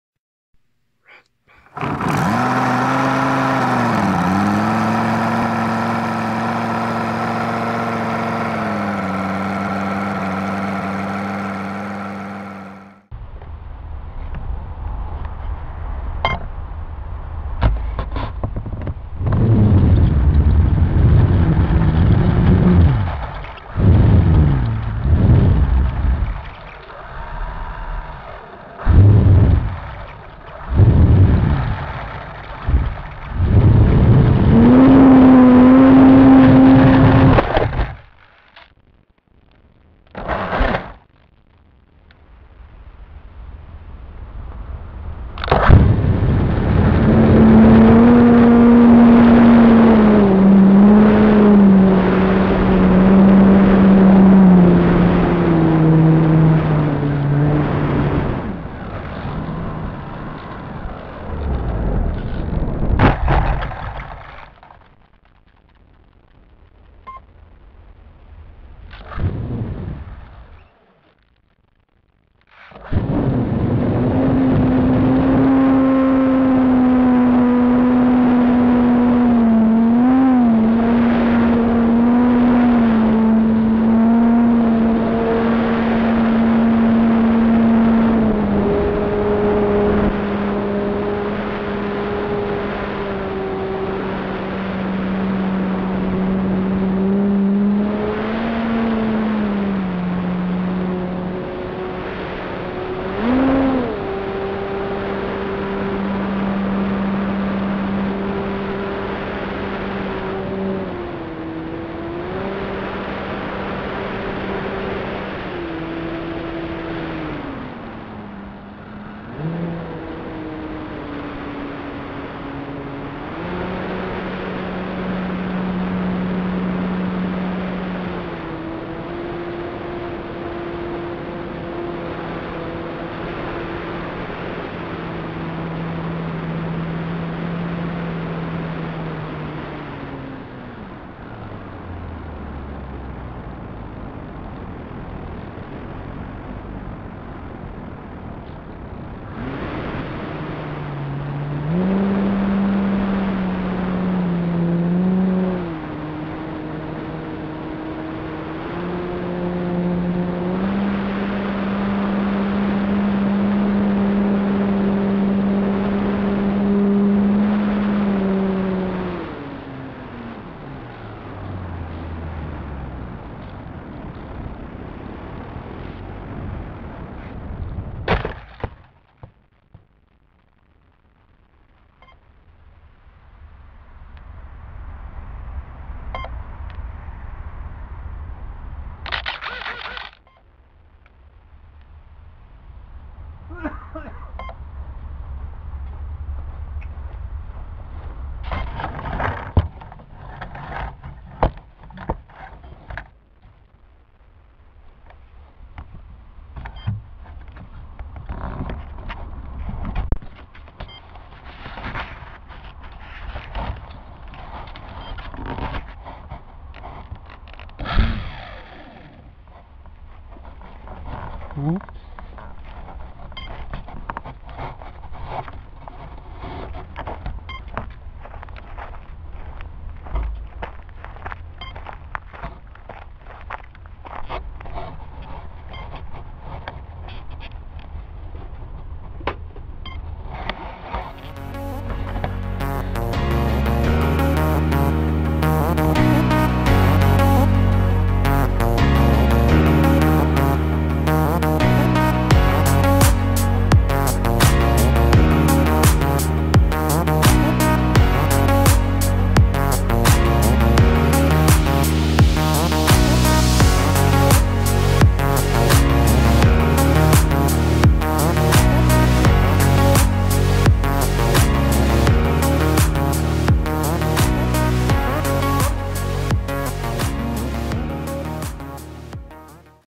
Joysway J3 Cub Sport with a crash.
Haven't been flying for a long time, so when i found my cub, i put hubsan FPV DIY radio on it.
For some reason the plane were behaving badly.
I have no clue if there's something with the radio, as i seem to remember the previous plane i had this one also behaved weird.
Oh well, the damage is minor and could have been field rep'd if i had glue and kicker with me.
More fun to come

and i own none of it.
This video are made for entertainment only and no revenue.